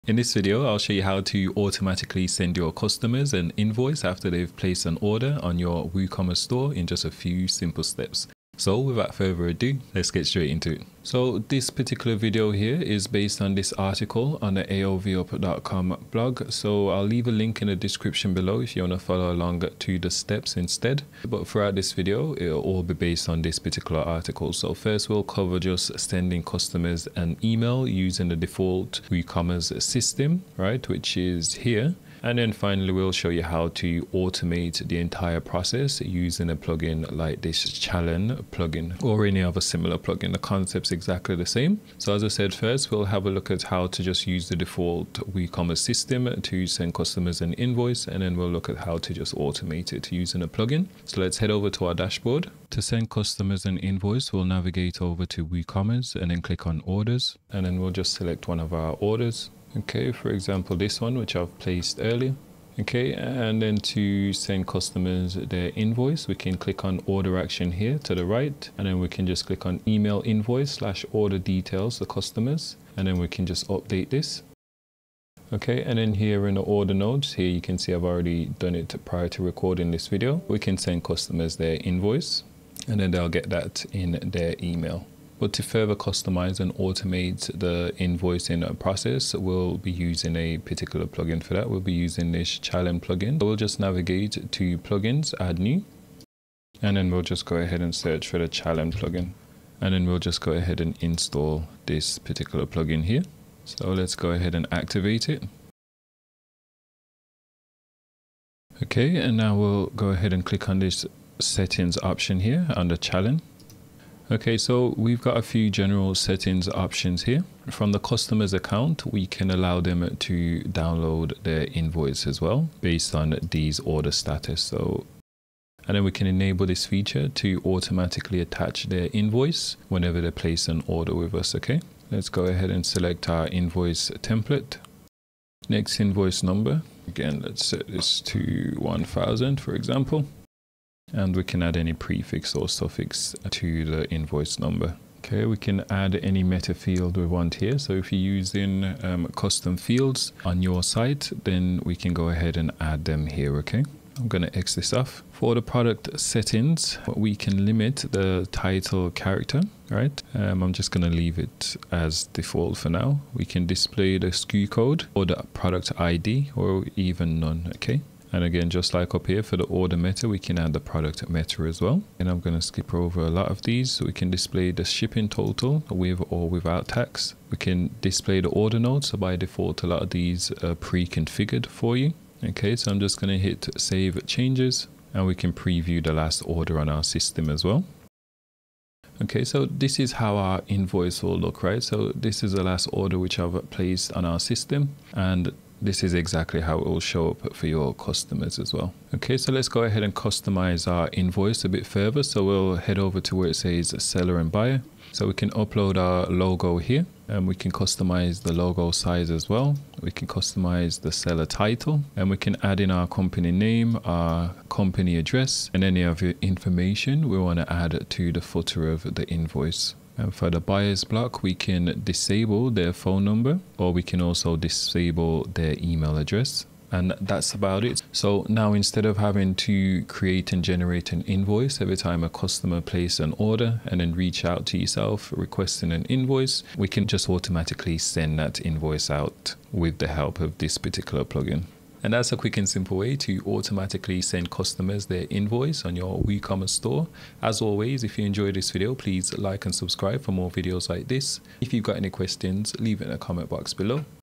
Send Invoice Automatically in WooCommerce
In this video, I'll show you how to send invoices automatically in your WooCommerce store.

00:00 Intro
00:10 Overview
00:52 How to Manually send customers' invoice
01:25 How to automate customer invoice
03:05 Test invoice
04:01 Outro